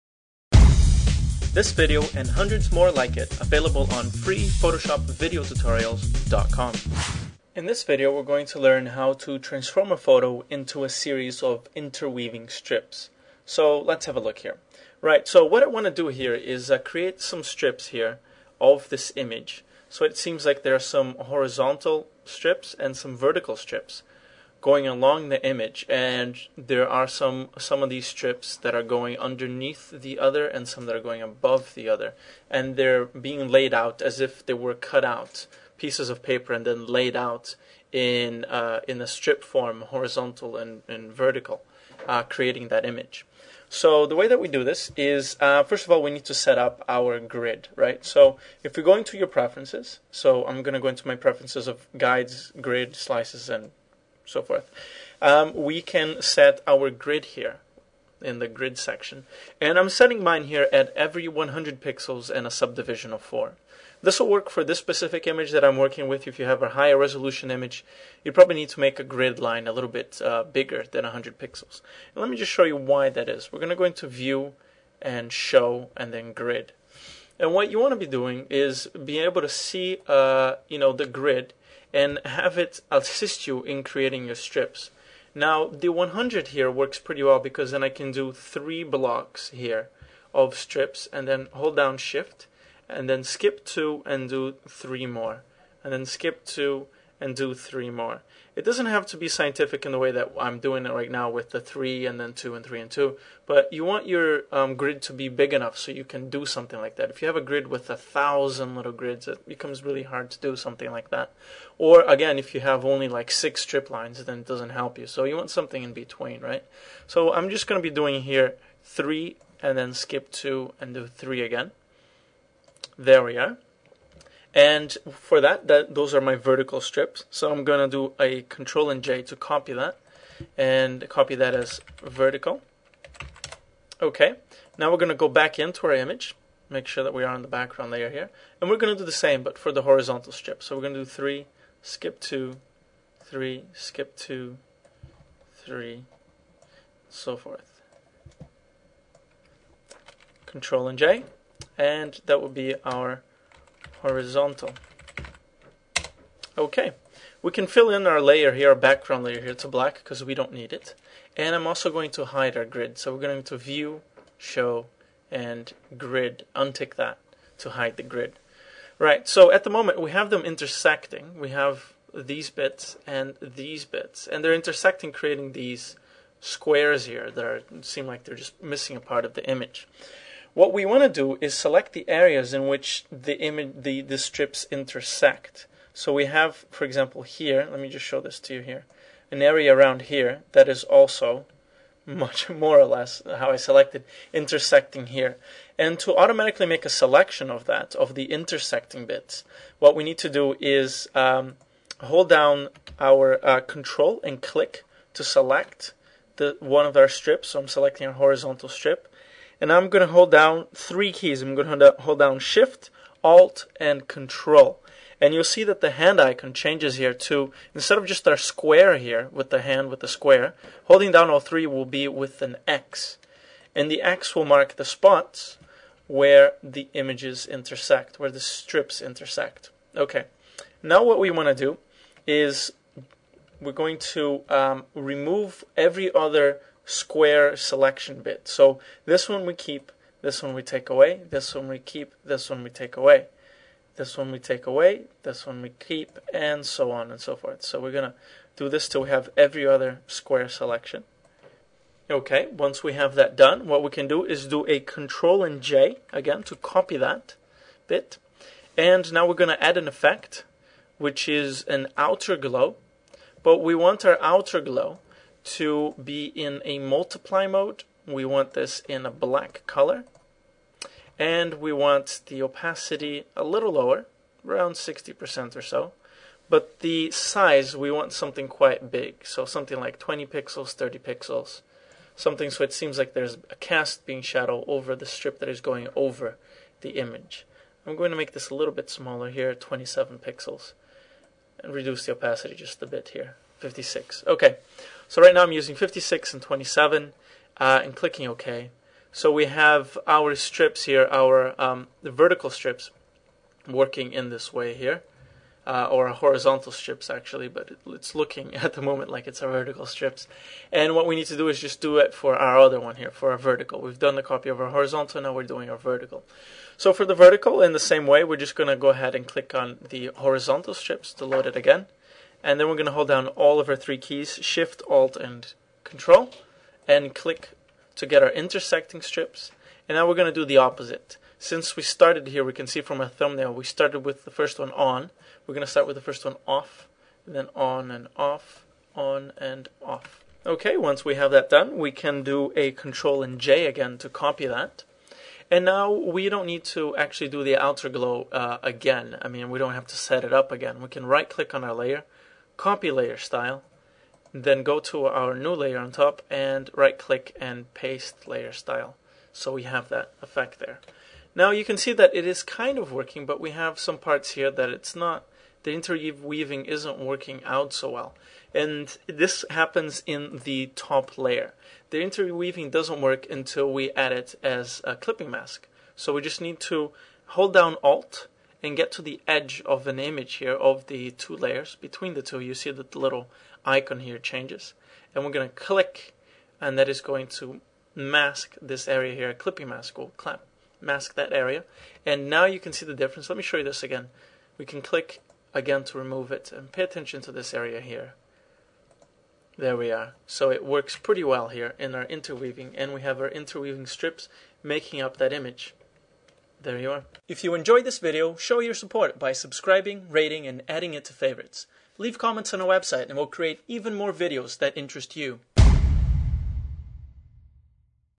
Learn Photoshop - How to Create Interweaving Strips
How to transform a photo into a series of interweaving strips. For this effect, we begin by defining a custom grid size. Vertical and horizontal strips are created by making multiple selections with the rectangular marquee tool. A custom selection is made and an outer glow layer style is applied to each of the two layers and finally a clipping mask is applied to complete the effect.